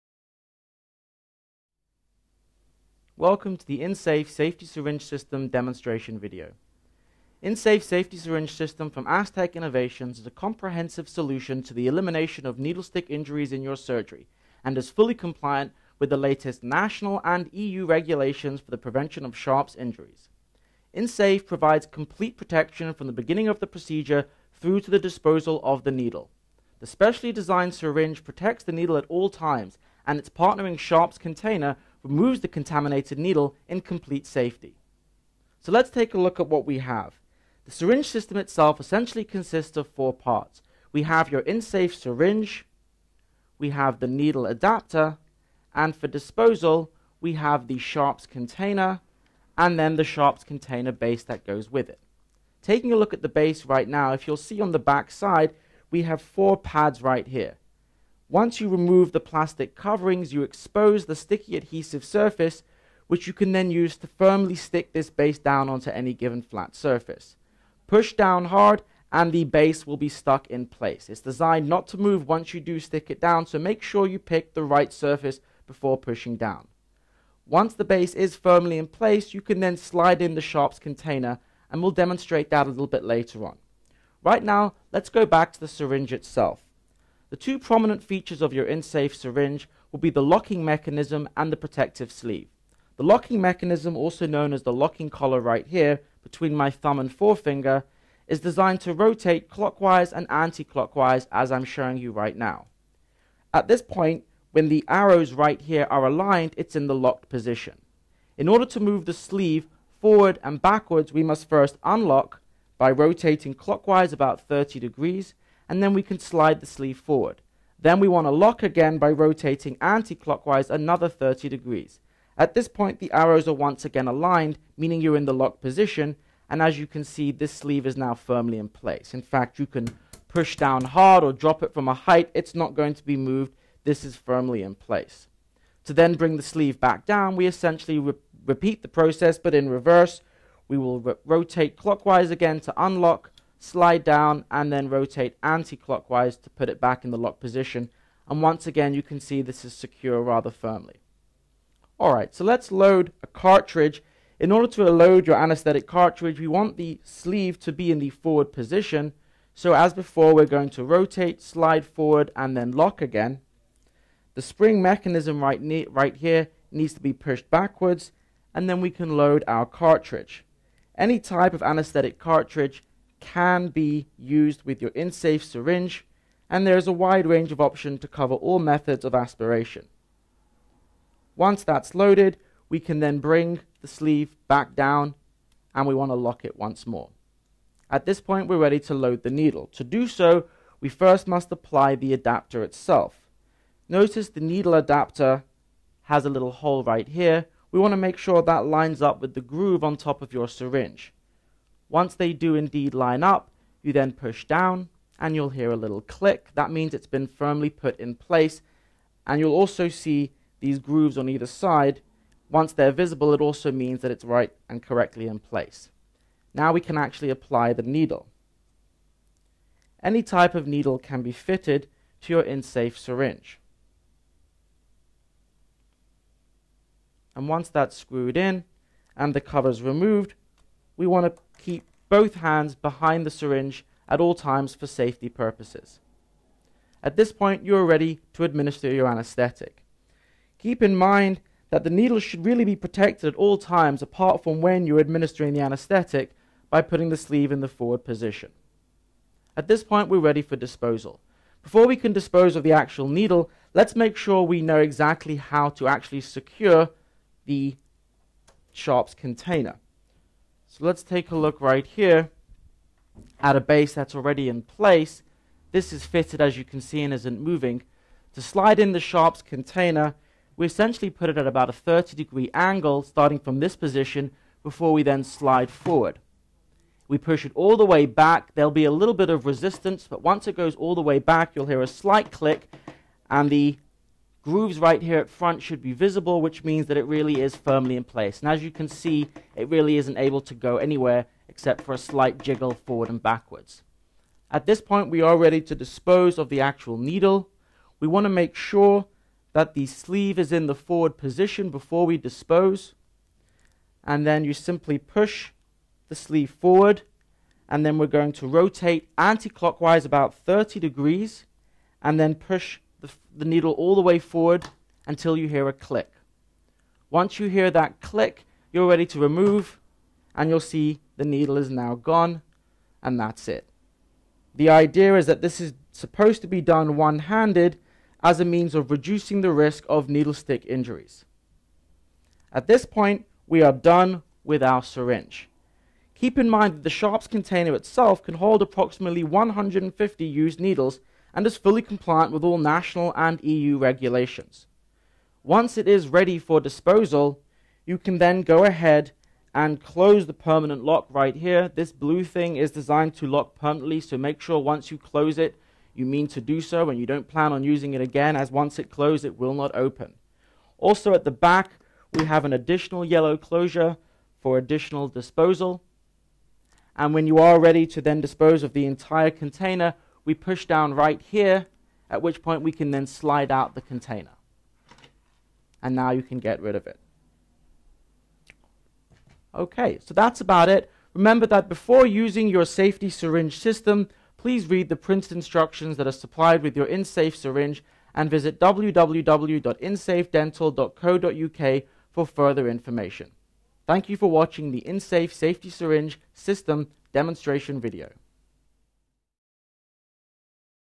InSafe Safety Syringe System Demo Video
inSafe provides complete protection from the beginning of the procedure through to the disposal of the needle